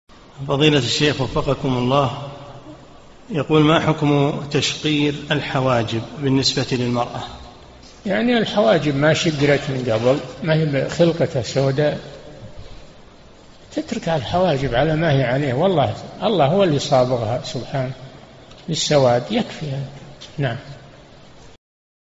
[640 -3022] ما حكم تشقير الحواجب بالنسبة للمرأة؟ - الشيخ صالح الفوزان
-"[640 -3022] السؤال: ما حكم تشقير الحواجب بالنسبة للمرأة؟
الجَوَابُ: يعني الحواجب ما شقرت من قبل ما هي بخلقة سوداء، تترك الحواجب على ما هي عليه، والله هو الذي صبغها سبحانه بالسواد يكفي هذا، نعم.
للاشتراك في قناة الشيخ صالح الفوزان

للاستفسار والإقتراحات والدعم
رئيس الفريق
لمشروع كبار العلماء